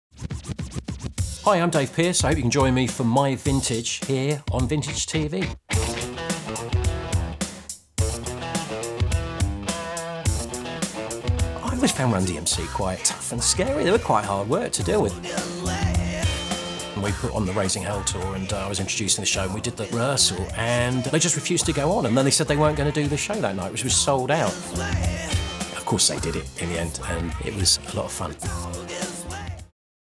Dave Pearce: My Vintage
Expect big anthemic floor-fillers and songs that pack a powerful punch from this influential, internationally-renowned British dance DJ and broadcaster.